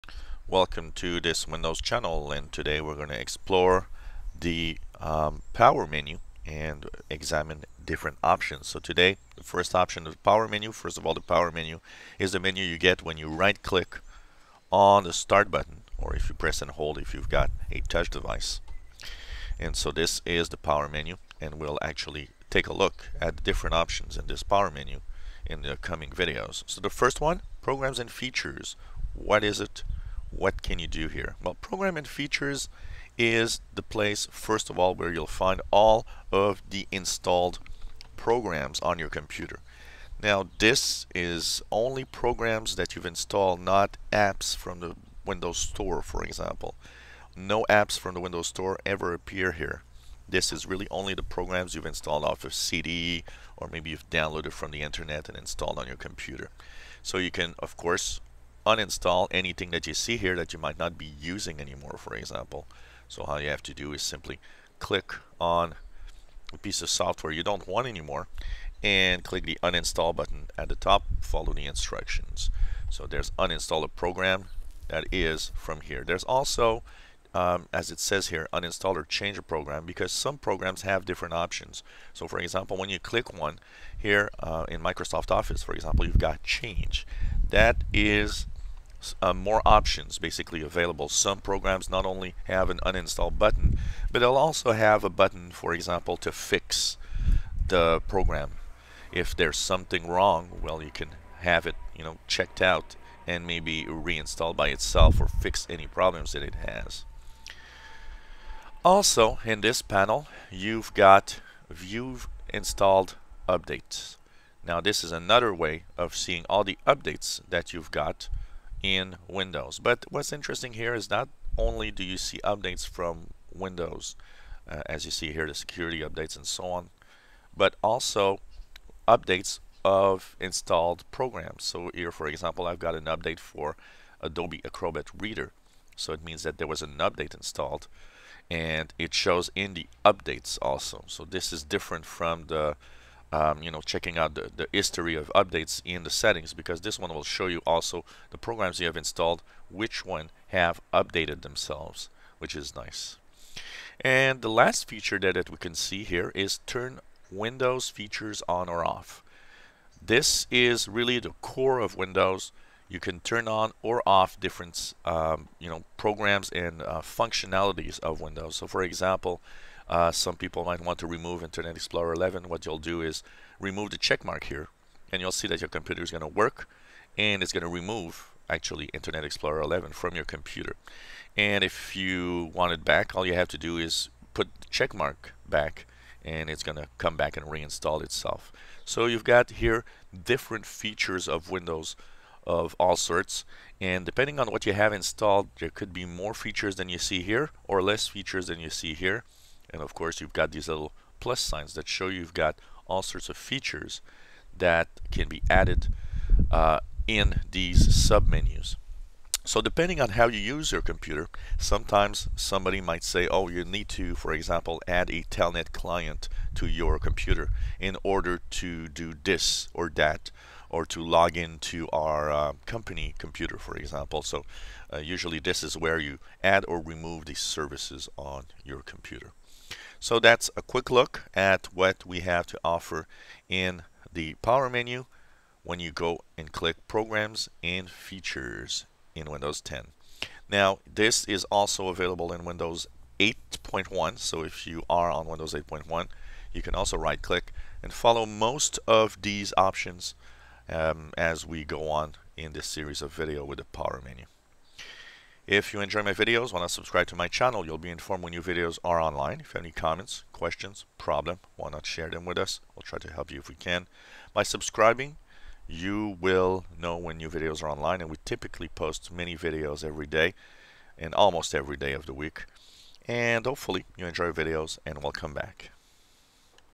Windows 10 Exploration Power menu Program and features option what it has to offer
In this series where we examine the start power menu we explain the options in Programs and features in Windows 10, this is also available for Windows 8.1 users